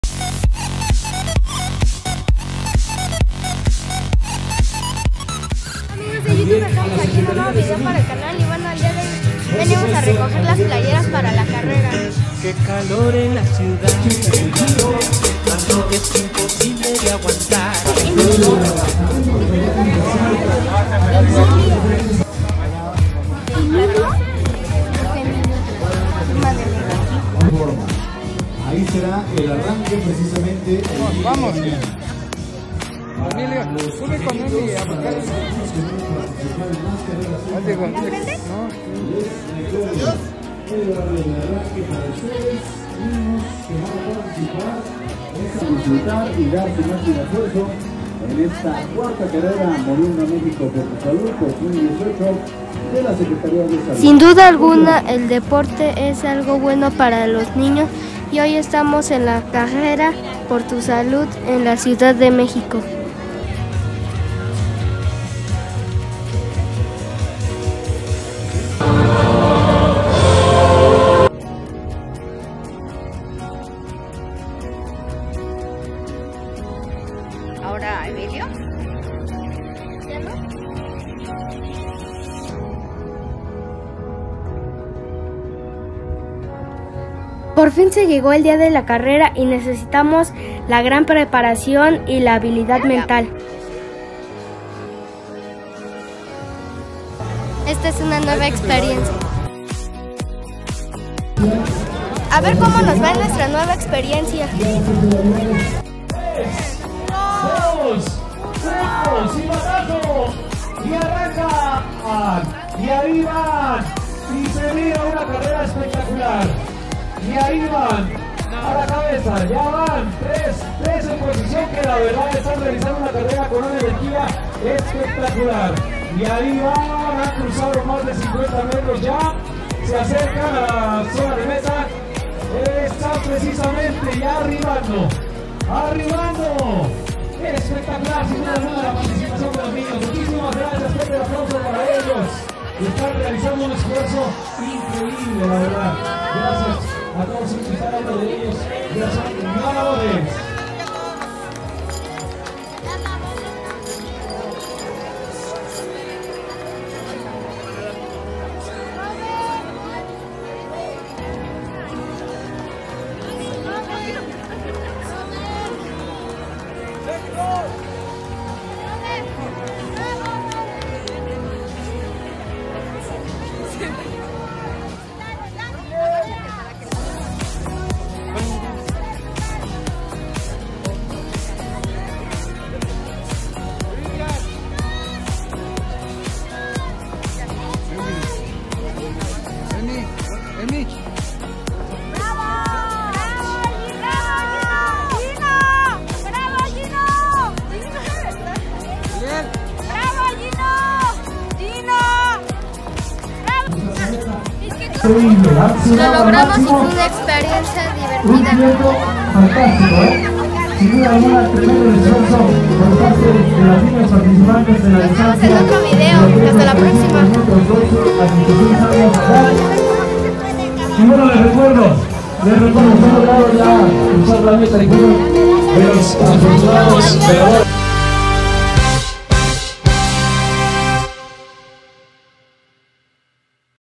A CORRER
La actividad física es parte de nuestra vida y el día de hoy con el gran reto logrado y no solo es la medalla y el escenario sino la satisfacción y el compromiso por empezar y seguir viviendo al máximo.
y comenta en que lugar crees que quedamos.
y da like